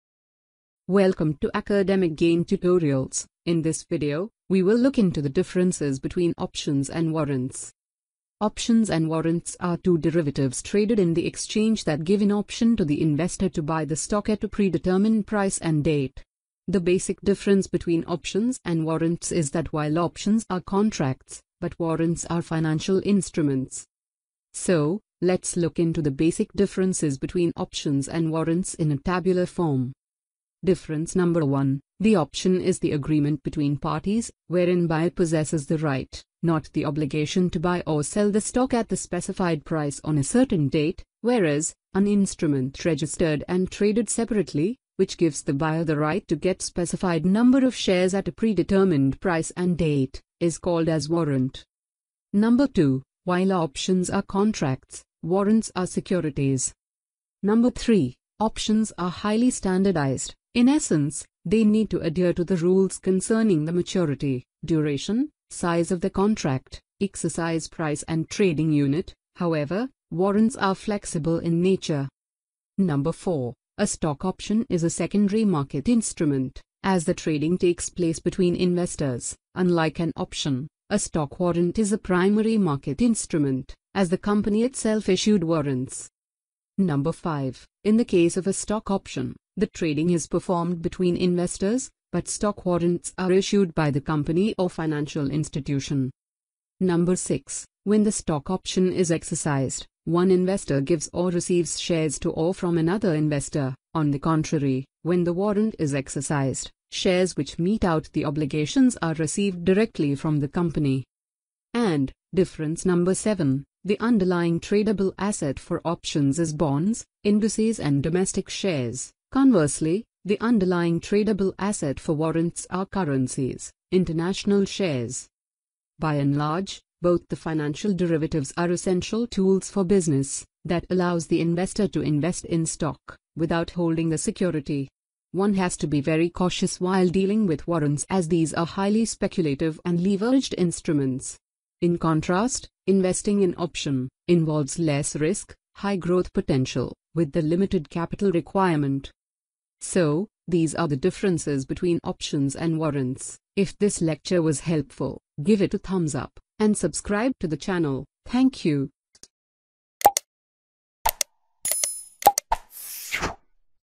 Differences between Options and Warrants.
This video covers a detailed discussion on the major differences between Options and Warrants.